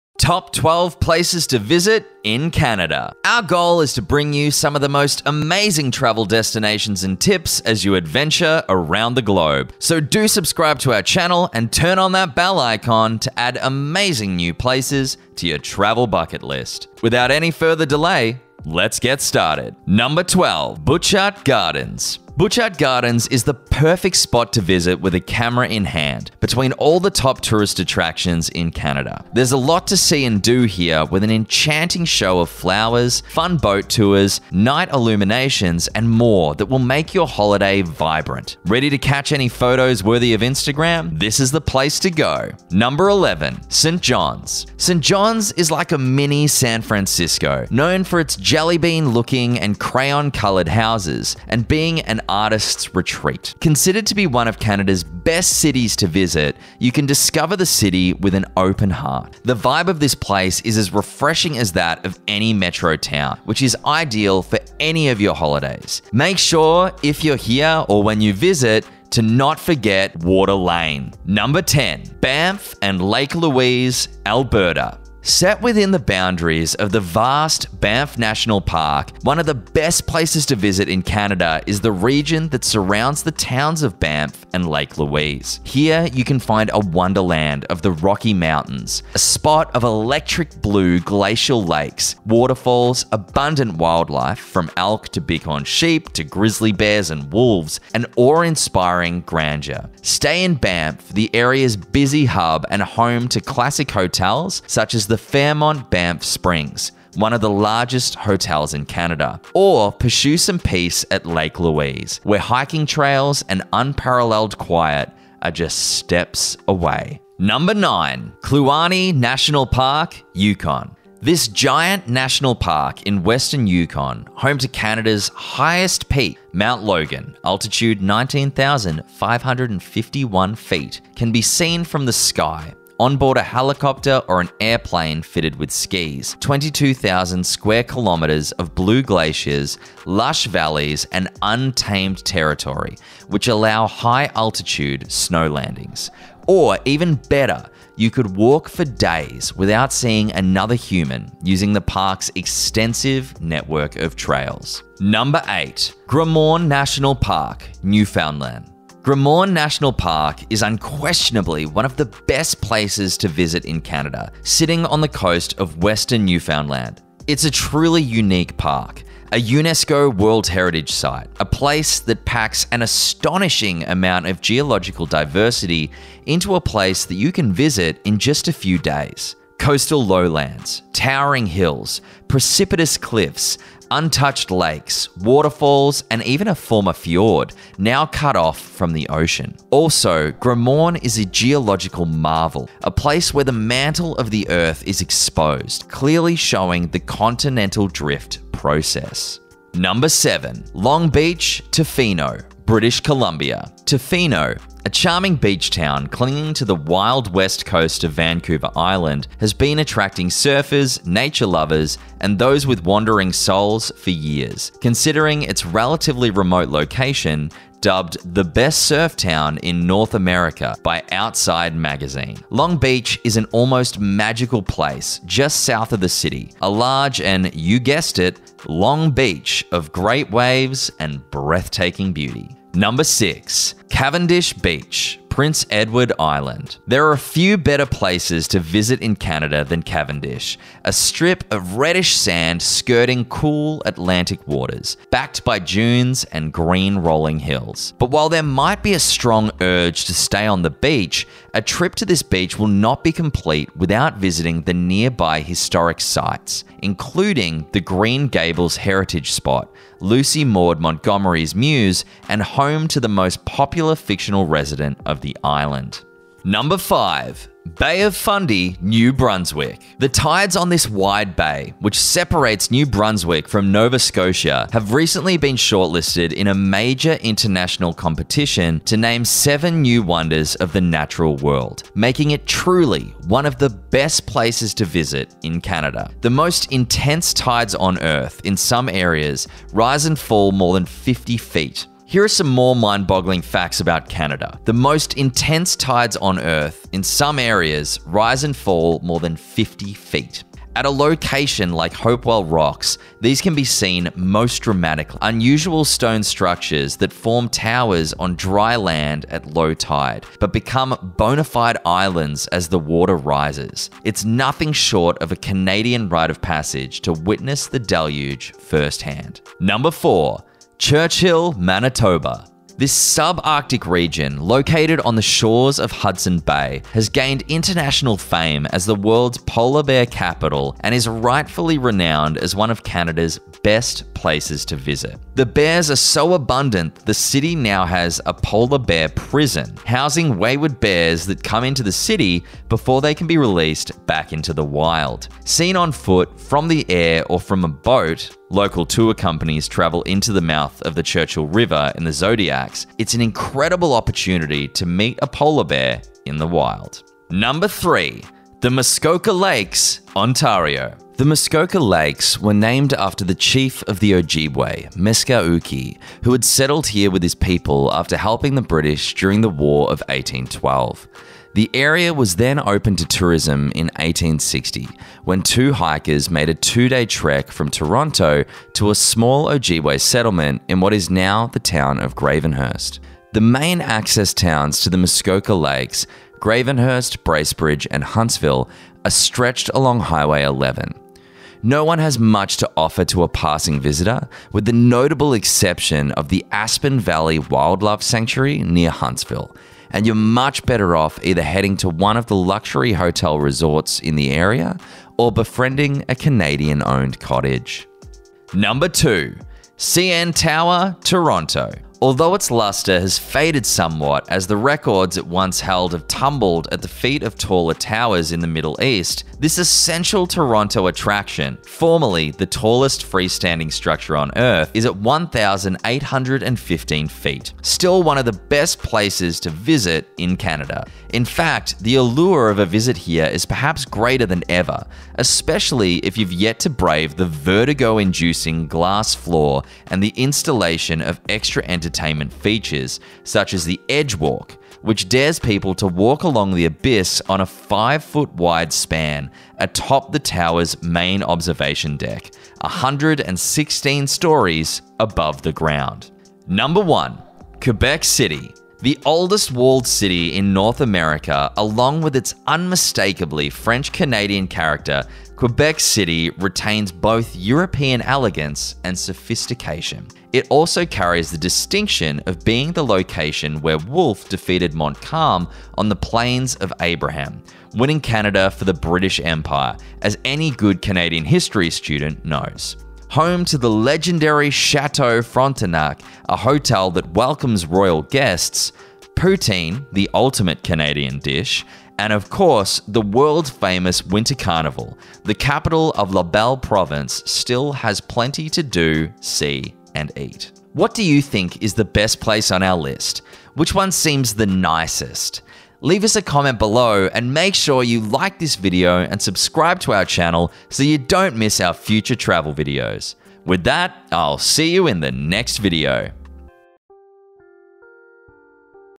Best Places to Visit in Canada
As the largest country in North America, Canada is a vast land of impressive landscapes and vibrant cities stretching across 10 provinces and 3 territories. Mostly British and French descent, Canada is home to a mosaic of multicultural communities. Today we explore the best places to visit in Canada. Which one are you most excited to visit?

We post weekly on Tuesday and Thursday.

Have a travel or food channel and want a shout out in one of our upcoming videos?